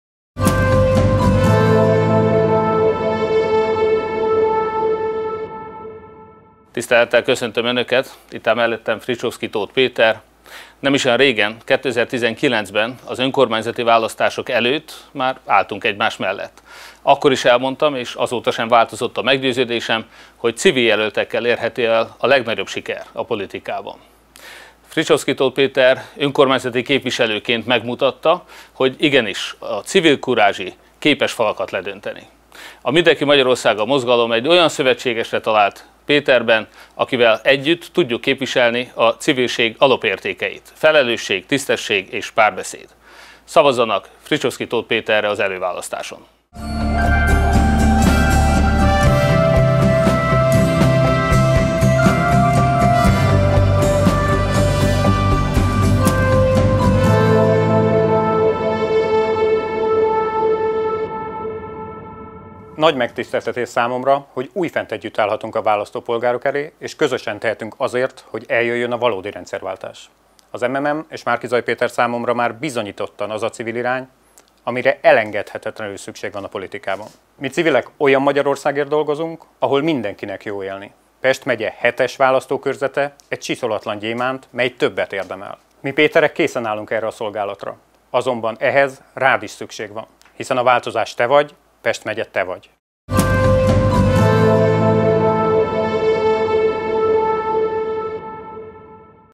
Fricsovszky-Tóth Péter bejelentése (Pest 7. OEVK)
A Mindenki Magyarországa Mozgalom Fricsovszky-Tóth Petert támogatja Pest megye 7-es számú választókerületében.

Nem is olyan régen, 2019-ben, az önkormányzati választások előtt már álltunk egymás mellett. Akkor is elmondtam, és azóta sem változott a meggyőződésem, hogy CIVIL jelöltekkel érhető el a legnagyobb siker a politikában. Fricsovszky-Tóth Péter önkormányzati képviselőként megmutatta, hogy igenis, a civil kurázsi képes falakat ledönteni.

Szavazzanak Fricsovszky-Tóth Péterre az előválasztáson!